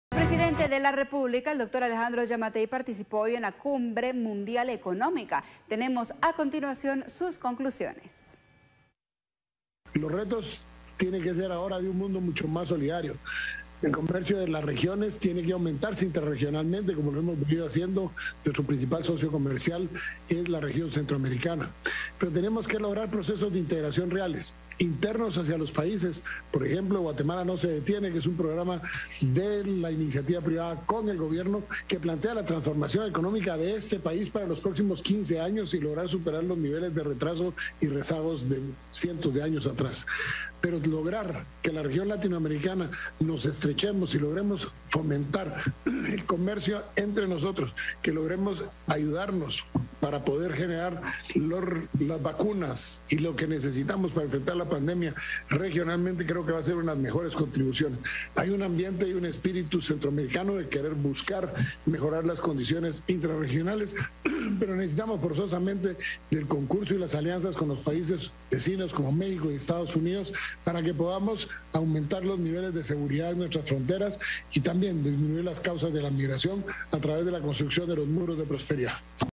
Presidente Giammattei participa de Cumbre Mundial Económica
Durante la jornada de este miércoles, el Presidente de la República, Alejandro Giammattei, participó de la Cumbre Mundial Económica de forma virtual.